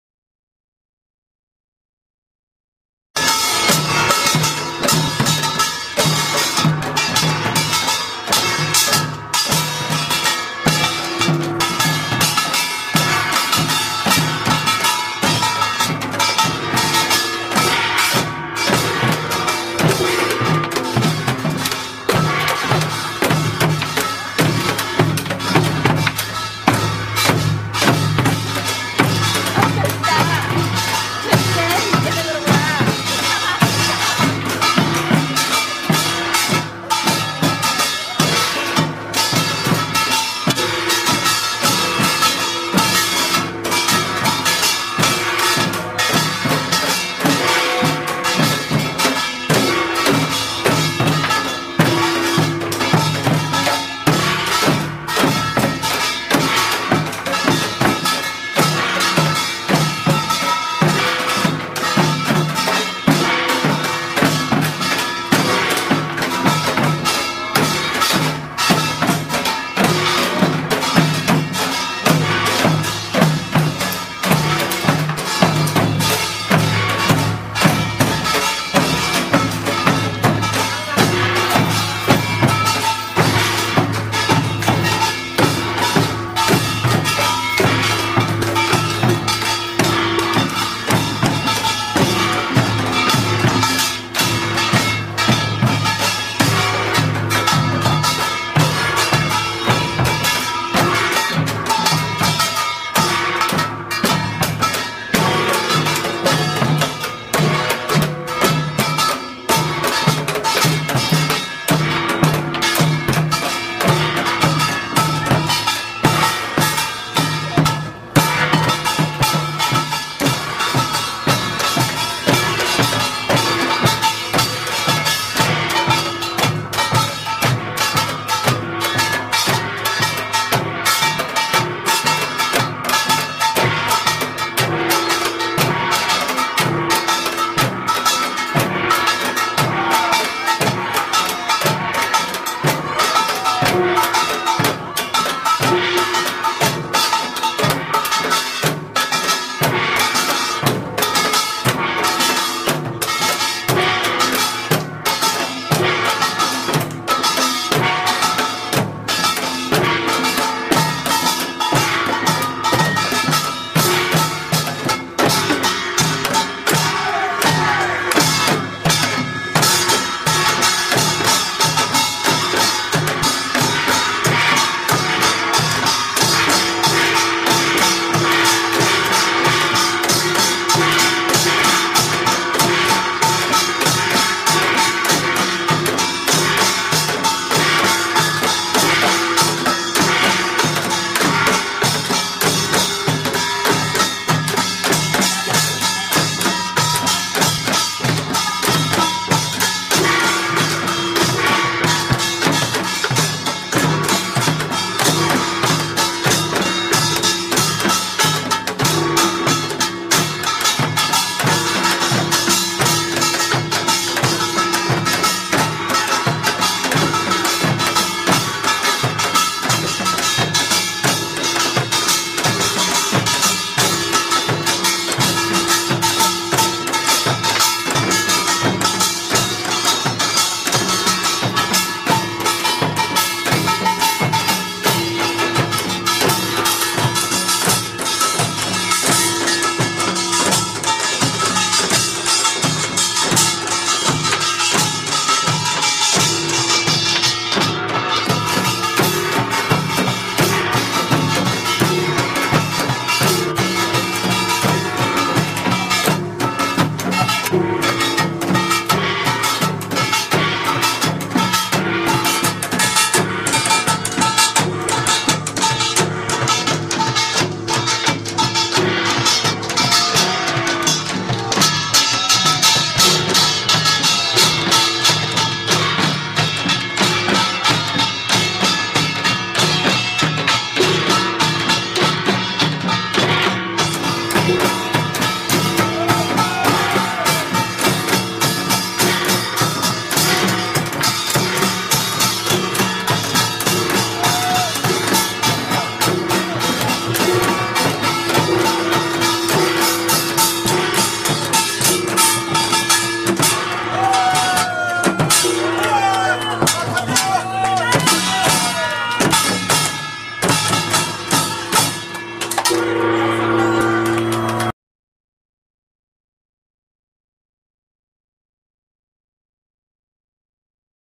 대전시 동구 용운동 풍물놀이 통영 야유회 에서
풍물놀이 마당 이영수 회장님과 단원과 함께